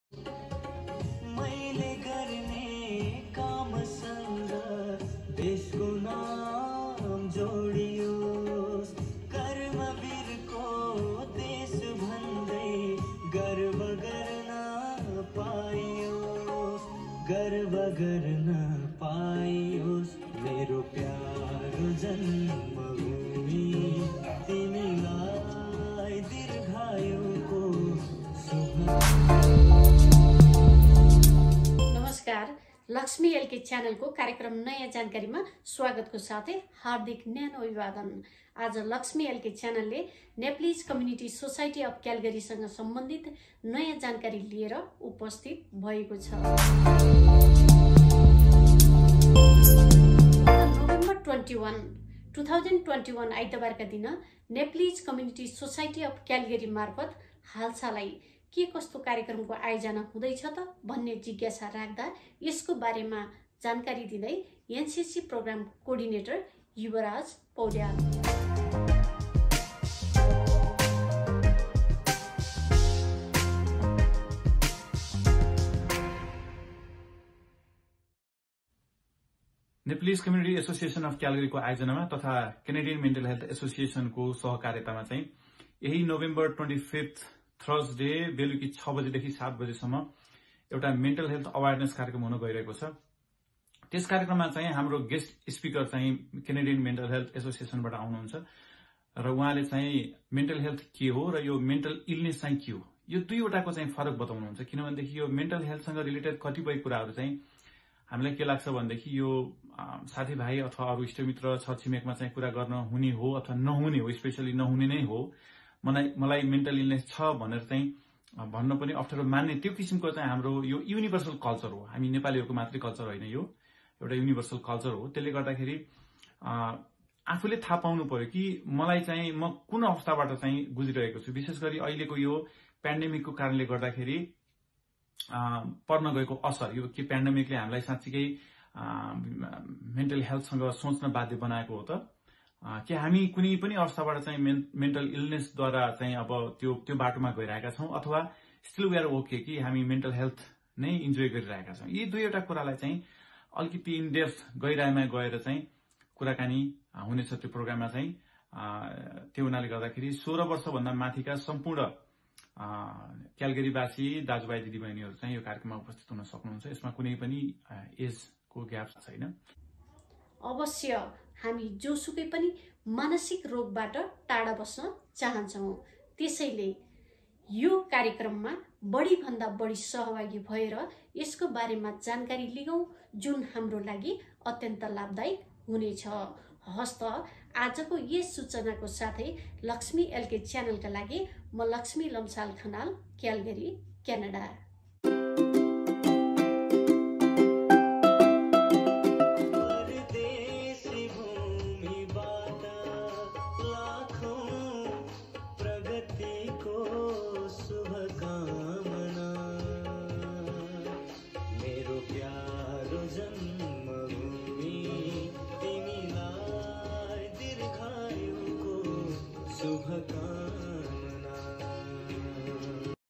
Mental Health Awareness Program ( NCSC/ NCAC)
अवश्य हामी जोसुकैपनि  मानसिक रोगबाट टाढा बस्न चाहन्छौ , त्यसैले Nepalese Community Association of Calgary ले आयोजना गरेको Mental Health Awareness Program मा बढि भन्दा बढी सहभागि भएर यसको बारेमा जानकारी लिगौ जुन  हाम्रो लागि अत्यन्त लाभदायिक हुनेछ र यस्तै नयॅा - नयॅा जानकारी लिनका निमित्त Laxmi LK channel लाई subscribe गर्नु भएको छैन भने subscribe गर्नुको साथै YouTube मा गएर लाईक, कम्मेन्ट र शियर गर्न नभुल्नुहोला।